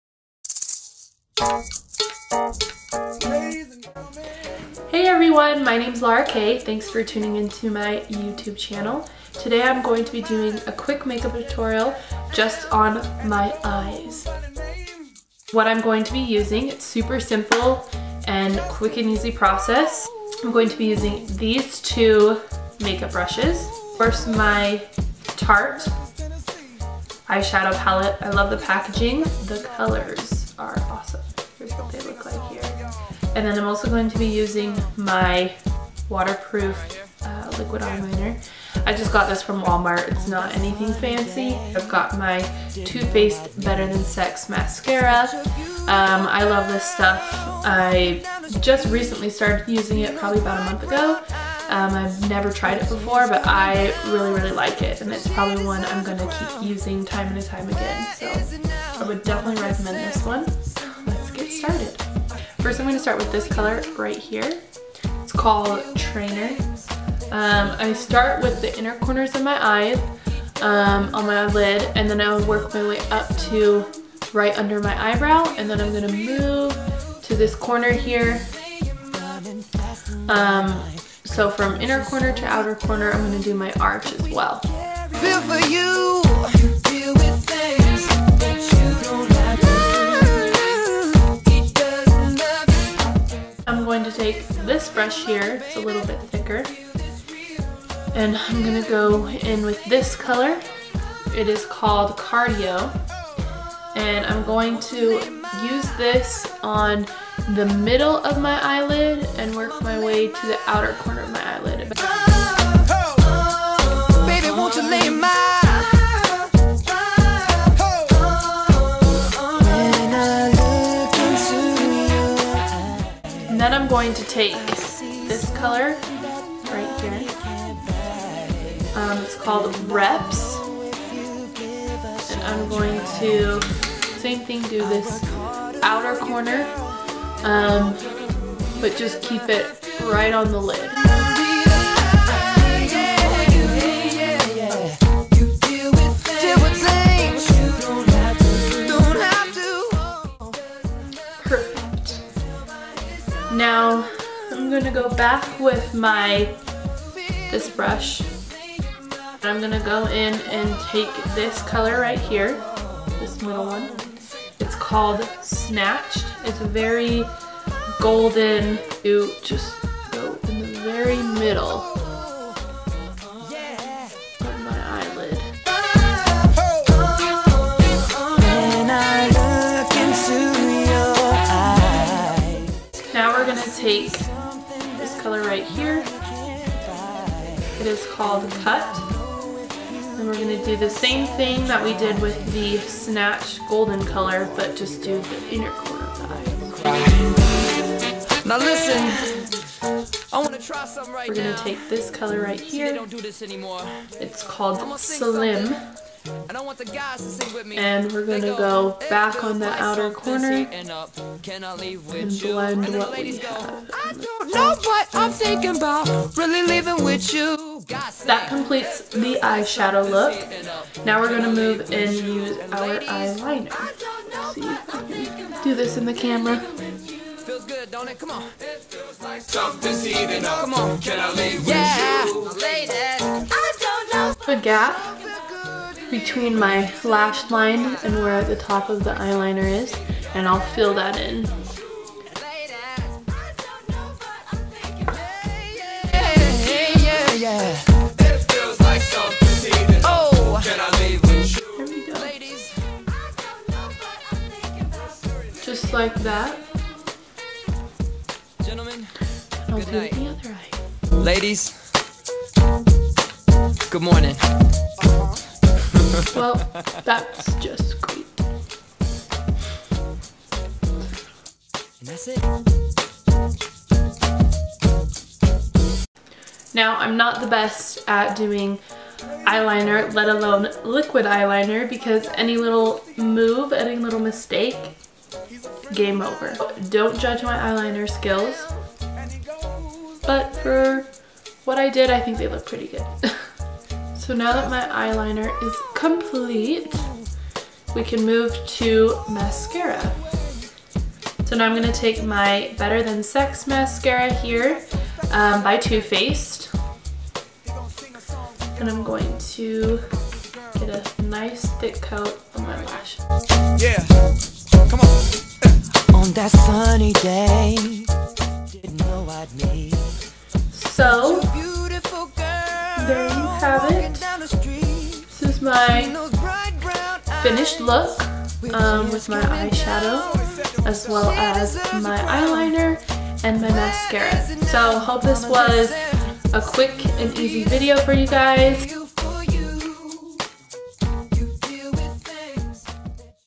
Kei Eyes Tutorial
Hello Everyone! Here is a quick and easy tutorial on how to do your eyes using the "shape your money maker eye & cheek palette" by Tarte.

Liquid Eyeliner:

Brushes: (Not the exact same as mine but pretty similar)

Please let me know your thoughts and if you have any questions!

xoxo

Larakei